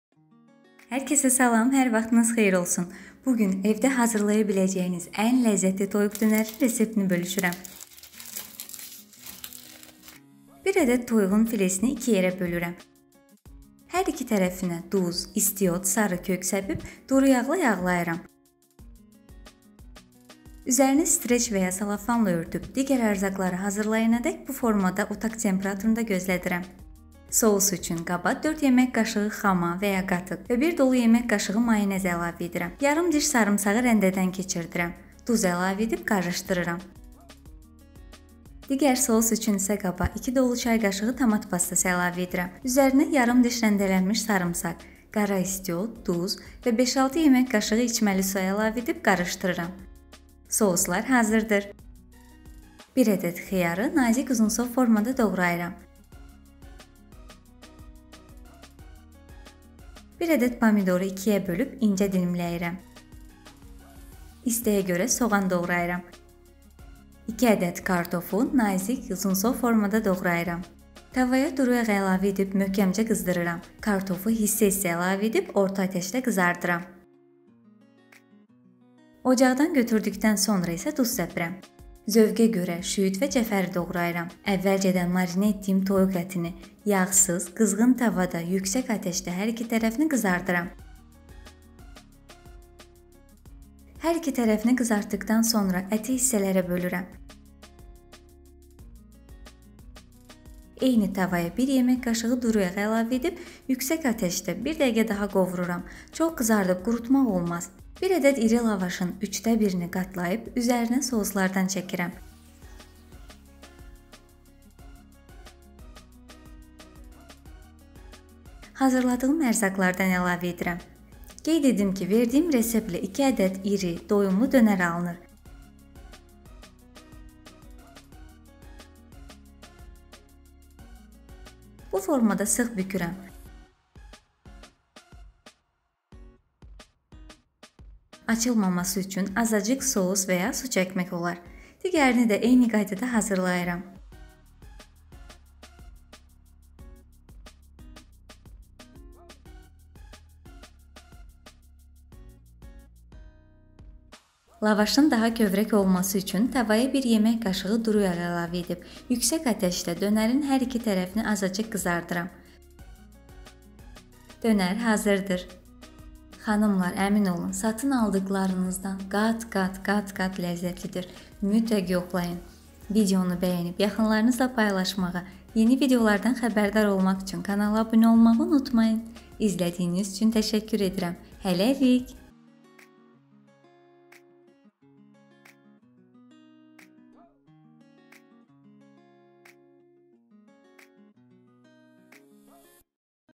EVDƏ ƏN LƏZZƏTLİ TOYUQ DÖNƏR HAZIRLANMASI
TOYUQ DÖNƏR RESEPTİ
Ərzaqlar
2 ədəd iri lavaş
1 ədəd toyuq filesi
1 ədəd xiyar
1 ədəd pomidor
2 ədəd kartof
Zövqə görə soğan
cəfəri, şüyüd
Sous üçün
4 yemək qaşığı xama və ya qatıq
1 yemək qaşığı mayonez
yarım diş sarımsaq
duz
2 dolu çay qaşığı tomat pastası
yarım diş sarımsaq
duz, istiot
5-6 yemək qaşığı su